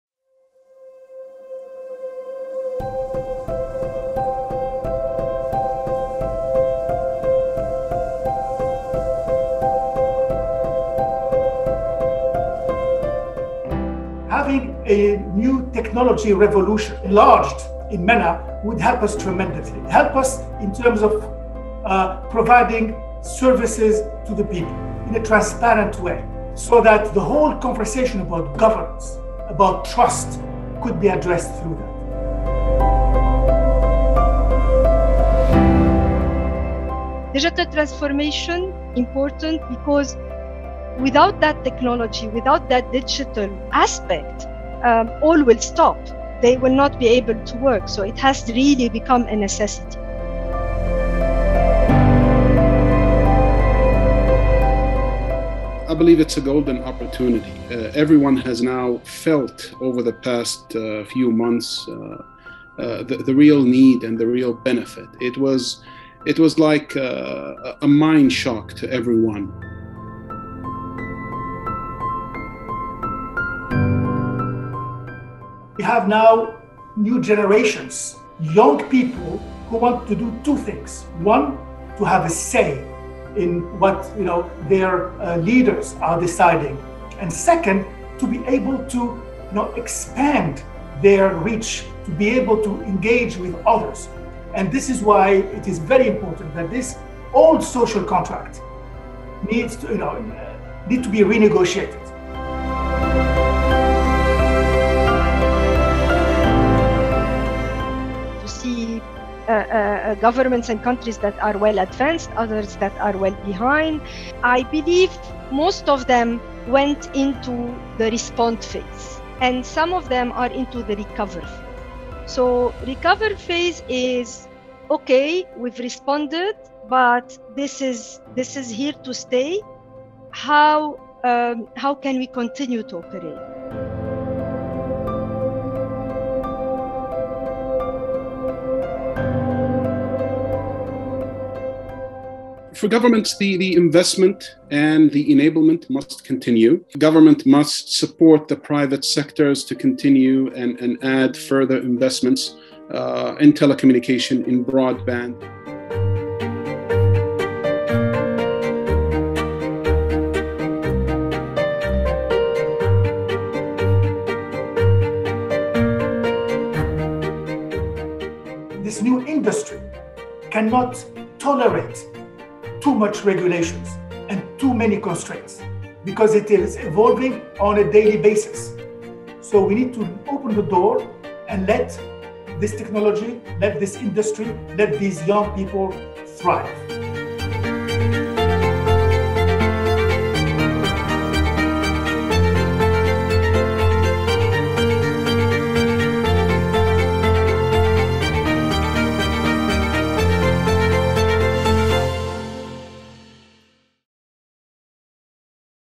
Middle East and North Africa Tech Initiative to help the region with digital transformation
Digital transformation became a widespread reality in Middle East and North Africa when COVID-19’s extreme social and physical distancing measures became part of each country’s emergency response to the pandemic. However, COVID-19 has also highlighted the impact of the digital divide and Middle Eastern and North African countries are still struggling with an unsustainable social contract. This can reversed if digital transformation is unlocked to promote trust, transparency, and accountability; assist governments to transform; and help create private sector led jobs.